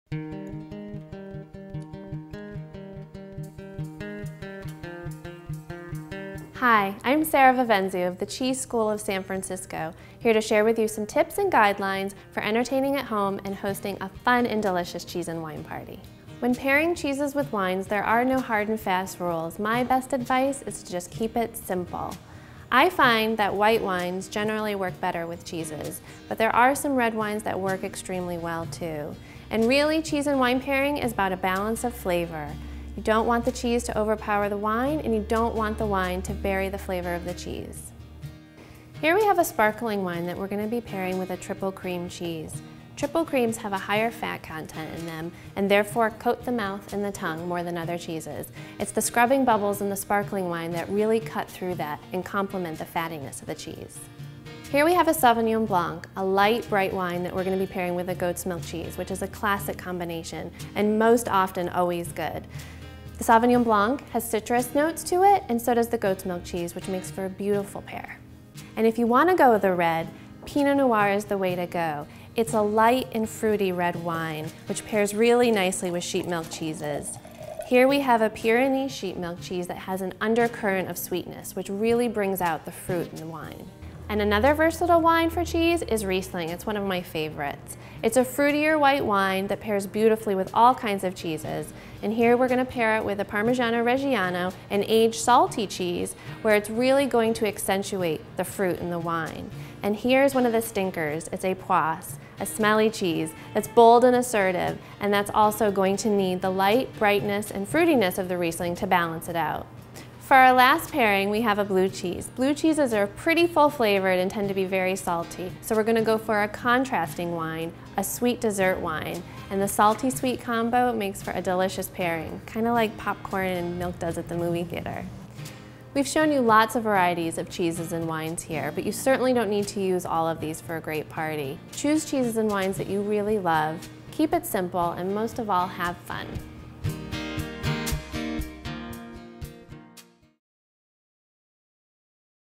How to pair cheese and wine | Pottery Barn
In this video, Sara Vivenzio of the Cheese School in San Francisco reveals some tips about pairing wine and cheese for entertaining at home.

There are no hard and fast rules for pairing cheese and wine, but she suggests keeping it simple.  According to her, white wines blend perfectly with cheese, but there are some red wines that work well with cheese as well. In her opinion, cheese and wine pairing is basically about balance of flavor.

Sara tells us that sparkling wine and triple crème cheese make a great combination. The triple crème cheese has a higher fat content and coats the mouth more than the other cheeses. The bubbles in the sparkling wine cut through it and compliments the cheese.

For red wine and cheese combination, Sara recommends Pinot Noir, as it is has a light and fruity taste, and can be served with sheep milk cheese. Another favorite of Sara is the Riesling wine. Riesling is a fruitier white wine that can be easily paired with all kind of cheeses. Sara pairs the Riesling wine with Parmigiano Reggiano and Stinker cheese. Parmigiano Reggiano's salty taste blends well with the Riesling wine, and the strong odor of the Stinker cheese compliments the light flavor of the Riesling wine.